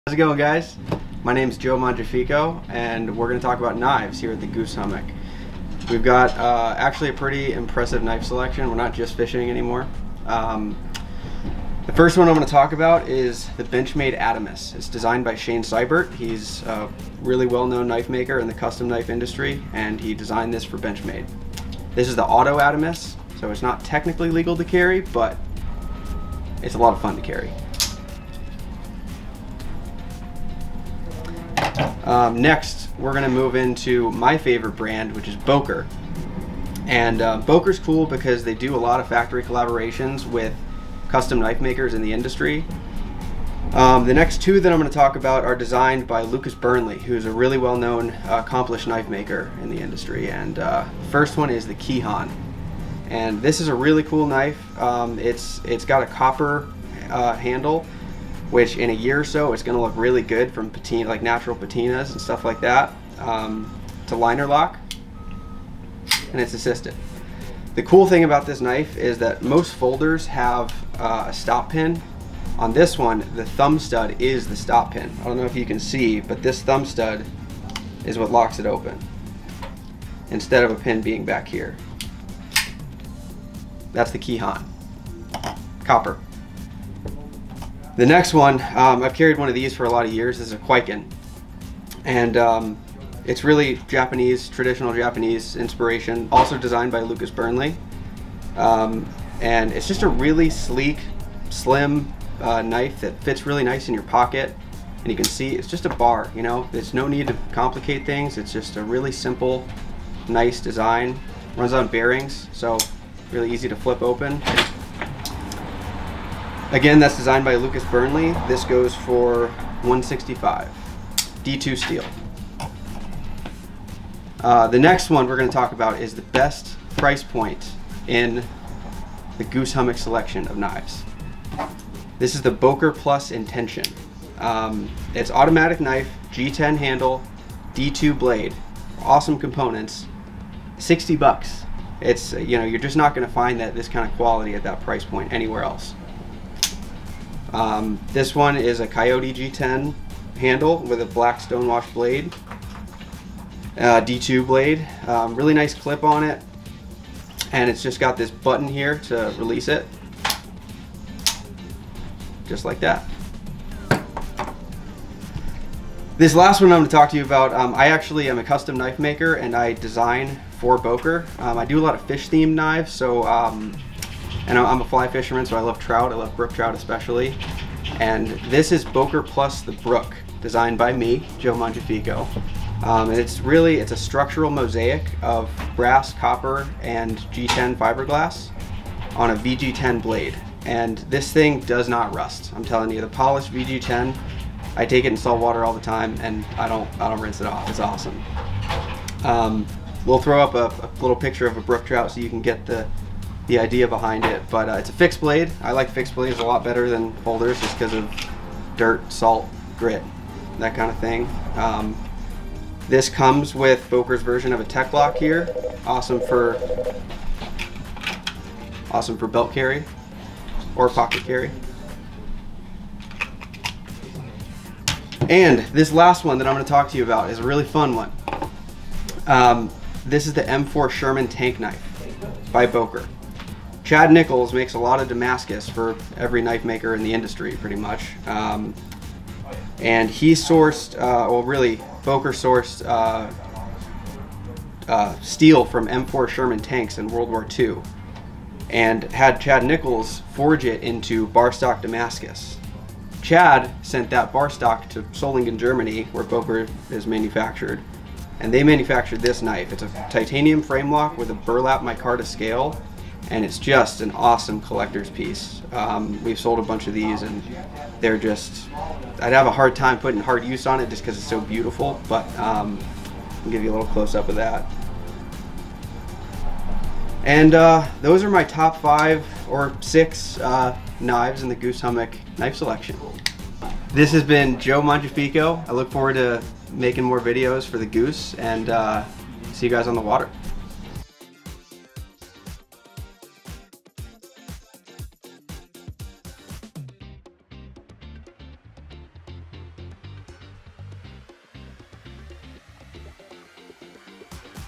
Top Knives this Month at the Goose Hummock
Joe is juiced up and ready to tell us about some of his favorite knives! We have had a bunch of new brands coming in and the one joe is excited for is Boker!

Check out and if you want to talk more about knives he is the man!